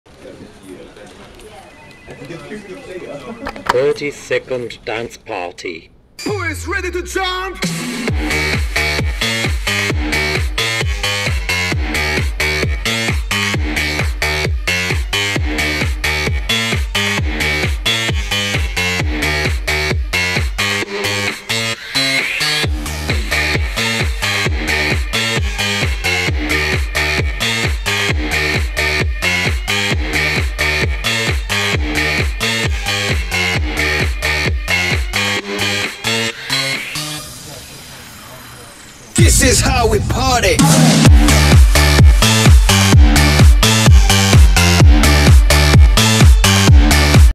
30 Second Dance Party - Volume II, Song 3 Demonstration
Nevin Derminguez shows us the proper way to break down to the all new button Vol II.